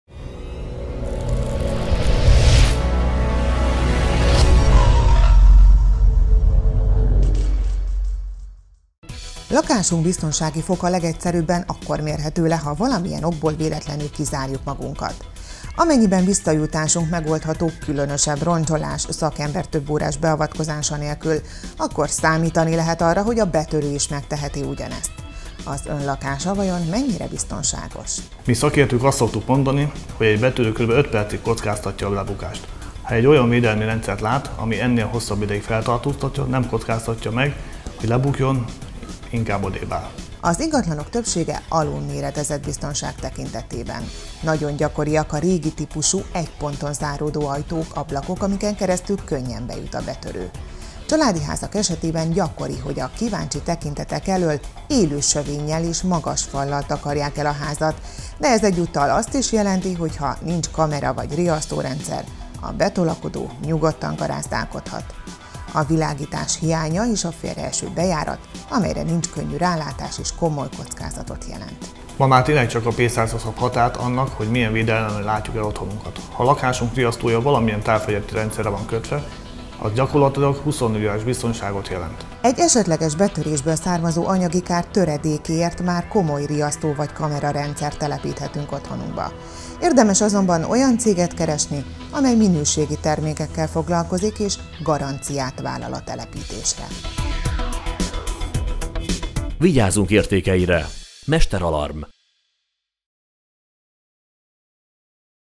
MesterAlarm - Vagyonvédelem mesterfokon
A gyors technikai fejlődés és a megbízható védelmi rendszerek megalkotása rohamosan az élő erő kivonását sürgeti a vagyonvédelemben. A biztonsági őröket elektronikus berendezések helyettesítik vagy erősítik. Ezt a dinamikus változást szolgálja szakértelmével a MesterAlarm Biztonságtechnikai és Vagyonvédelmi Kft., mely 2011 júliusában alakult felkészült szakmai háttérrel és kiemelkedő technikai felszereltséggel. Részletek